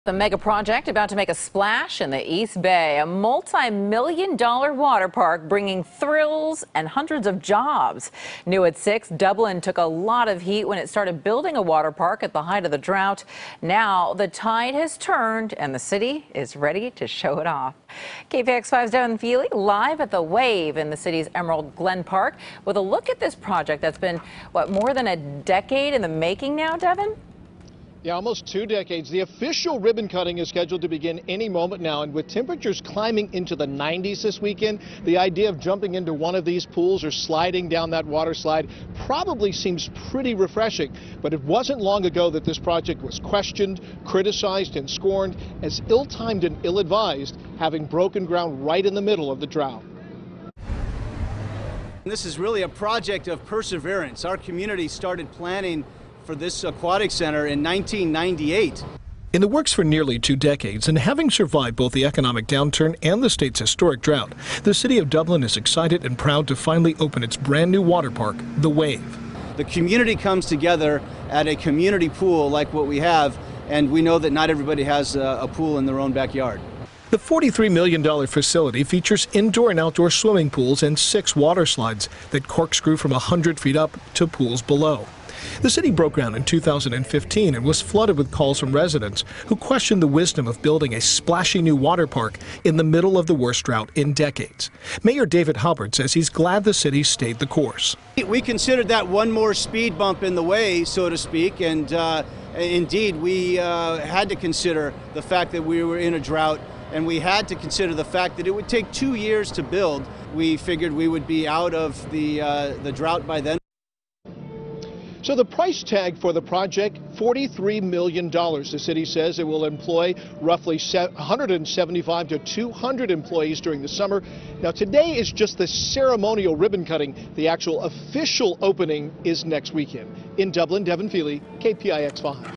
Mega Water Park Project Set To Open In East Bay
Devin Fehely takes an early look at a multi-million-dollar water park about to bring thrills and hundreds of jobs to Dublin. (5-19-17)